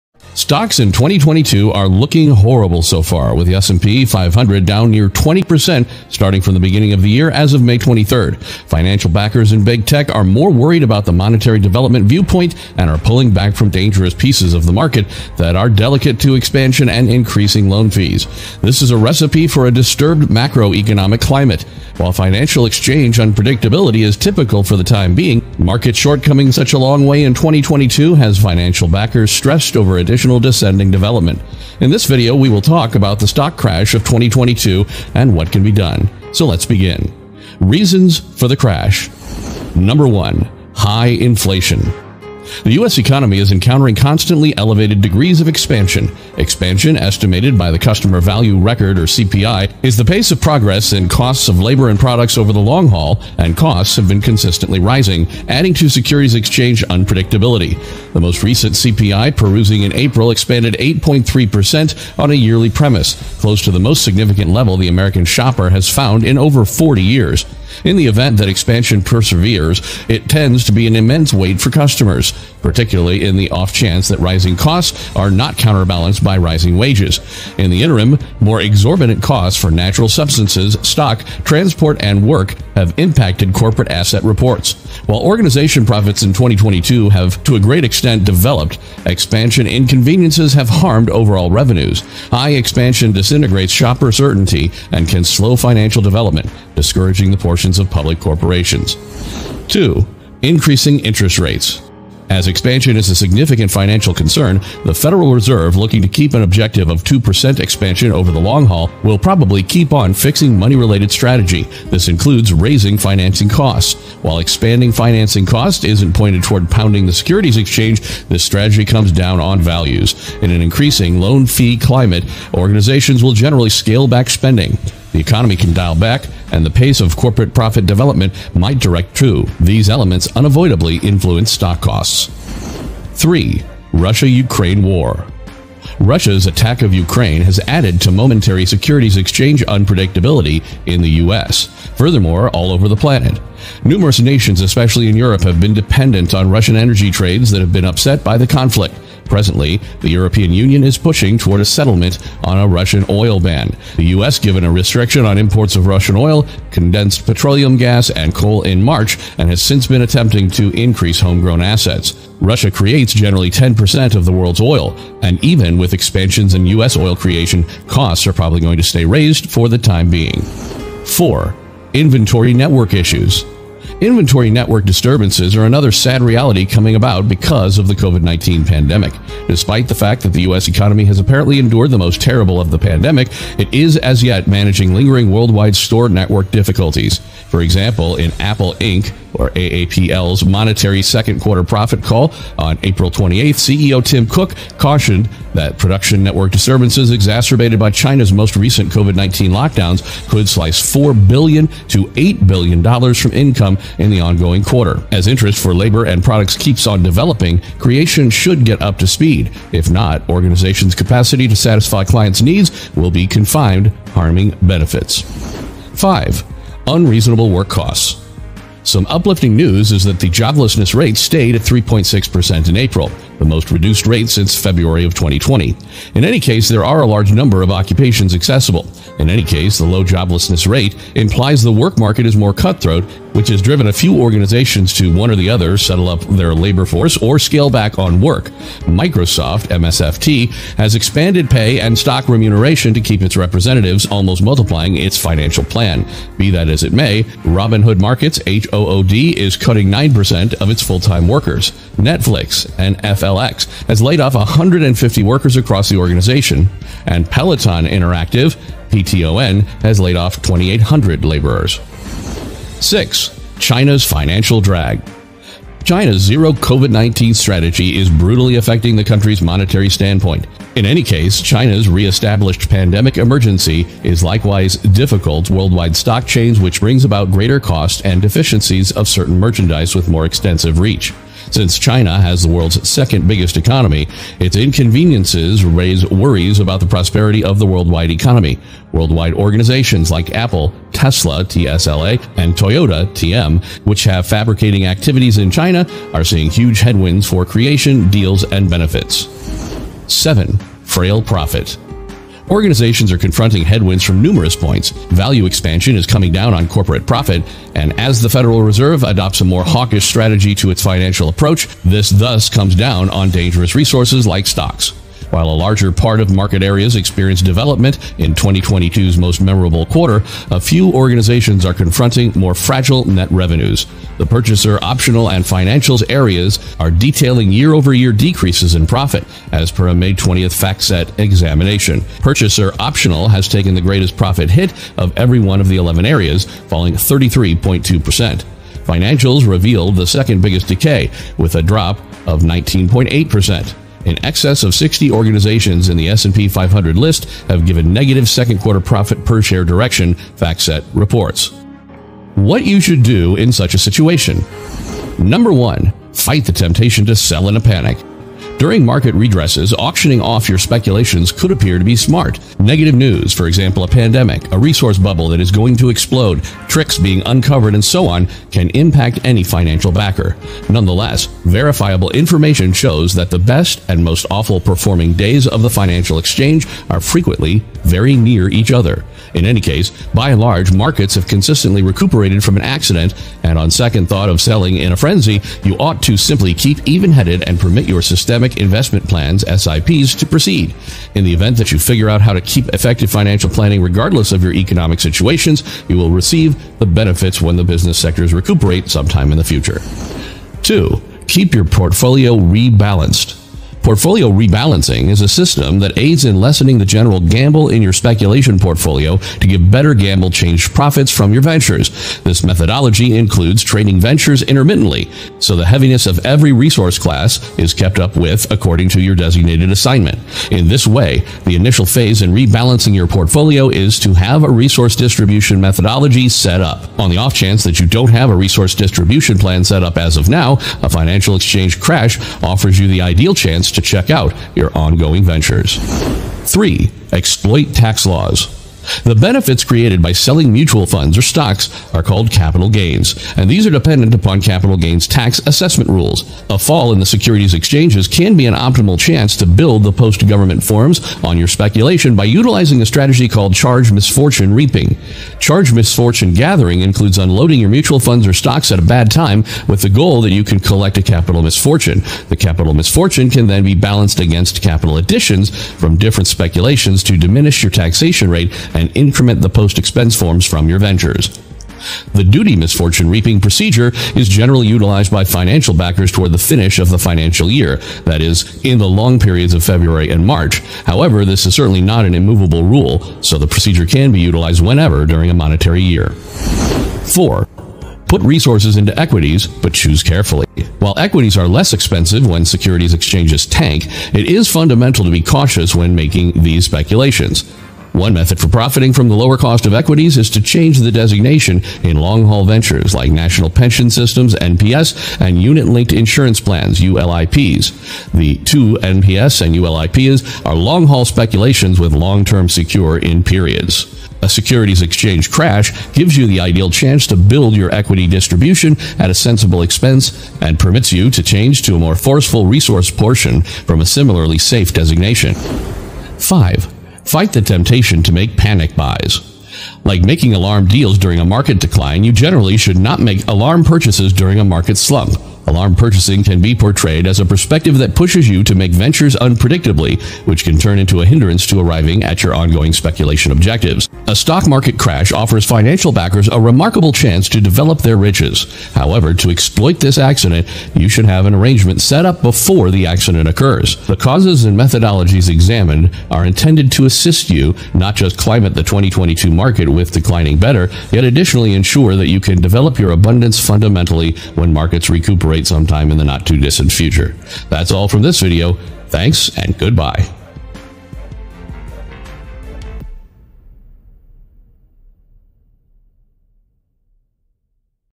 Stock Market Crash 2022 |What You Need To Do Now!!!
In this video i am giving you some tips that you need to do in the stock market crash of 2022